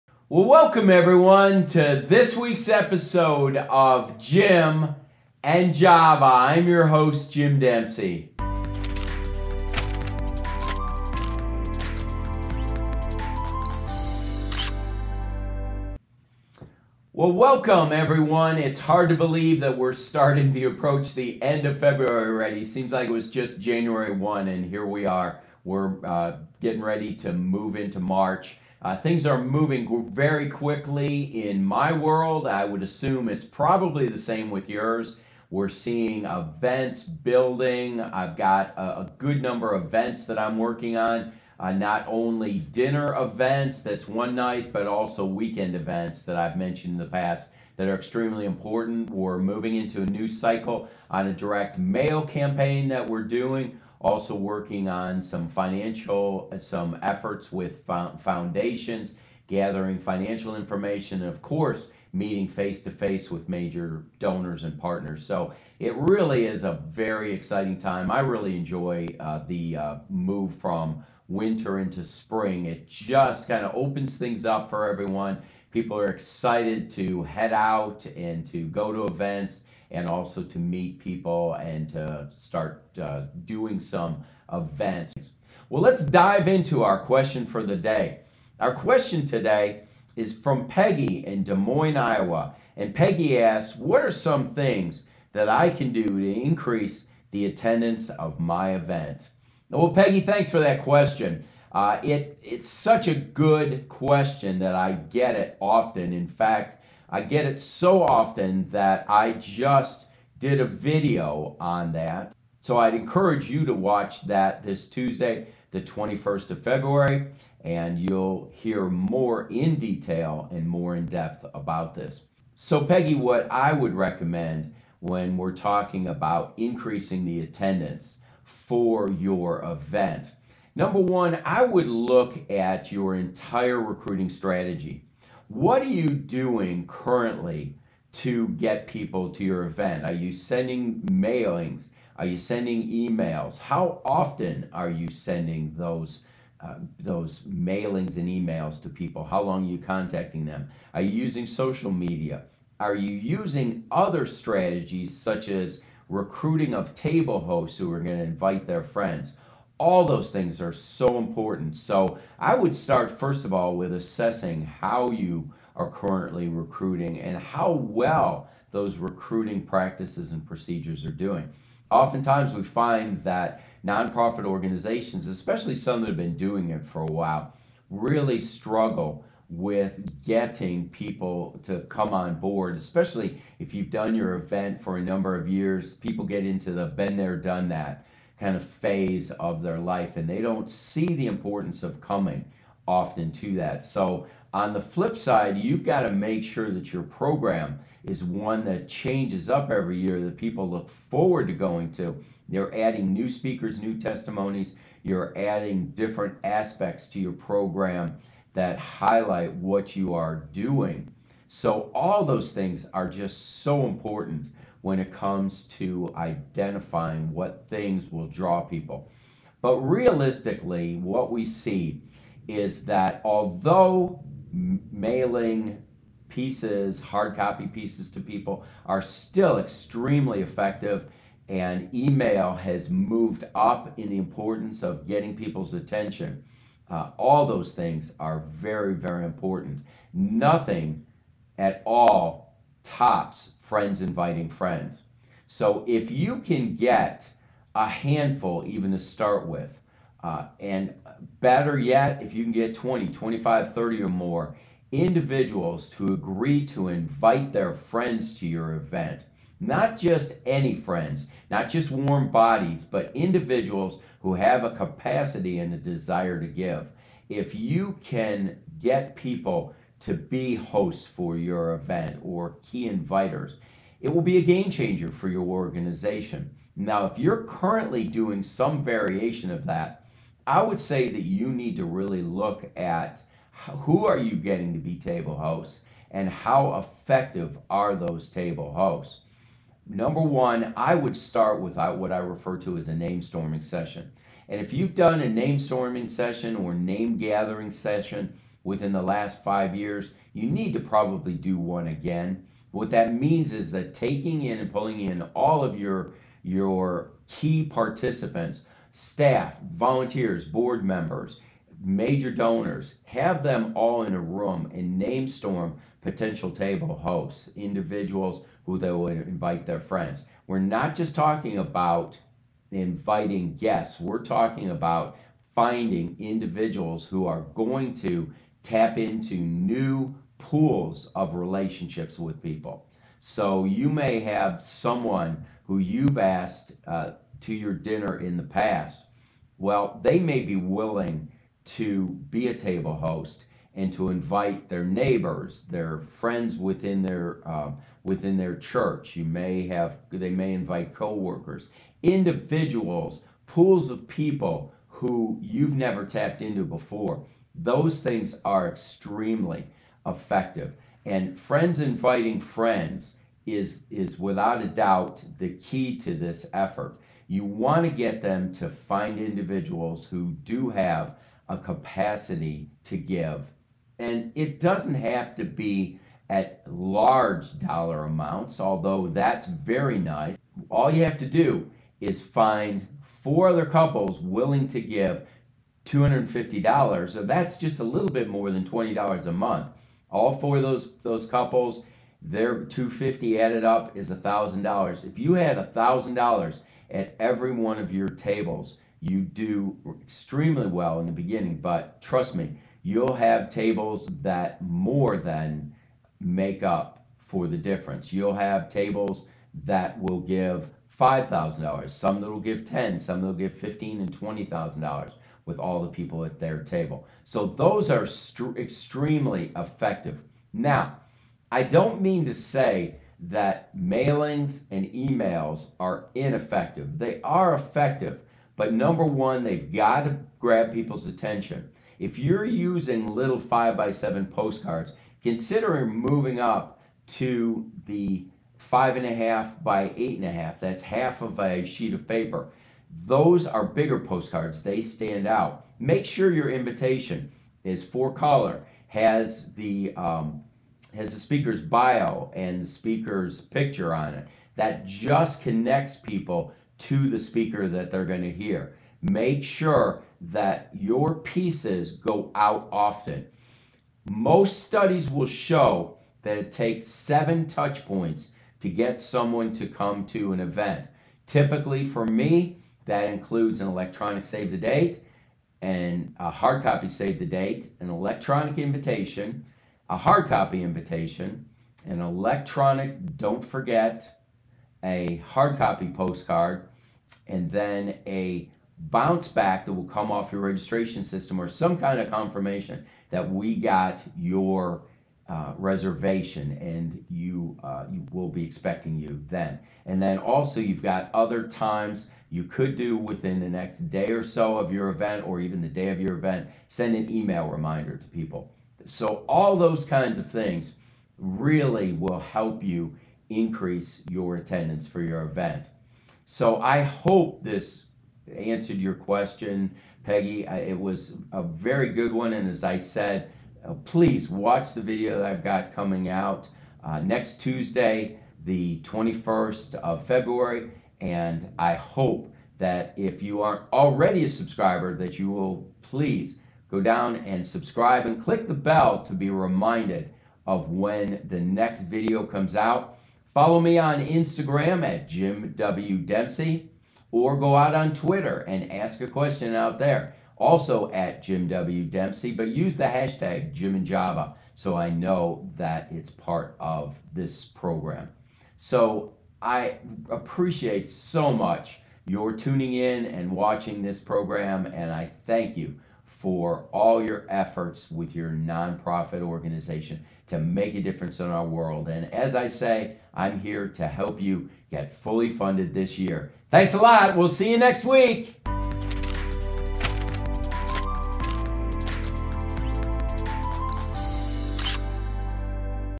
Jim 'N Java S3 E7: Tips and Q&A For Nonprofit Leaders | Nonprofit Fundraising
➡️  Jim 'N Java is a weekly broadcast designed to answer YOUR questions regarding fundraising for nonprofits.  If I select your question, it'll be answered on the following Saturday broadcast.

Timeline:
00:00  Introduction
0130  "What are things that I can do to increase the attendance of my event?""
08:50  Action Steps